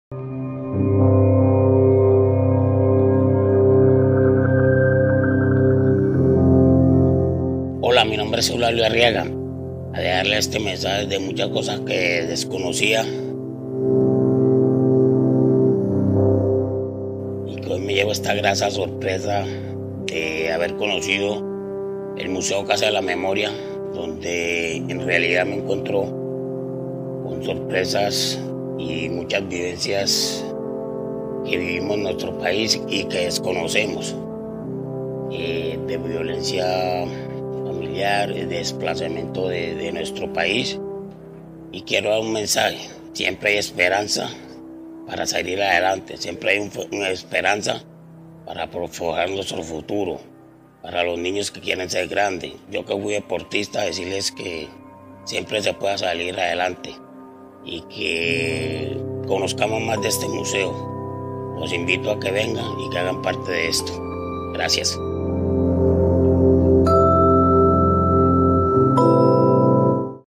⚽️ Entre goles y memoria | Eudalio Arriaga
🕊️ Hace algunos días nos visitó el exfutbolista Eudalio Arriaga y reflexionó sobre la necesidad de reconocer las realidades de nuestra sociedad.